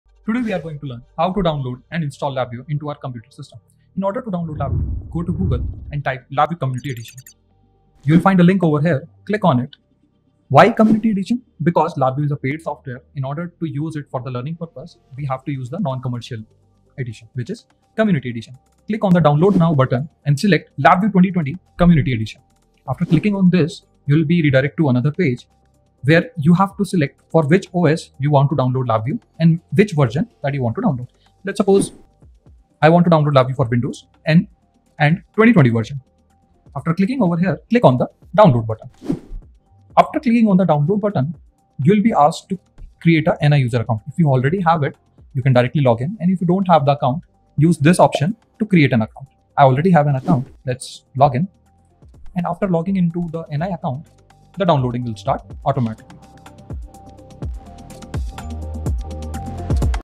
Learn how to Download and Install LabVIEW | Exeliq LabVIEW Beginner Course | LabVIEW Basics
In this video, we will learn how to install LabVIEW step by step. For proper installation watch the video till the end and don't skip any part. Let's start this Exeliq Tech Talks journey. Be smart and Code fast.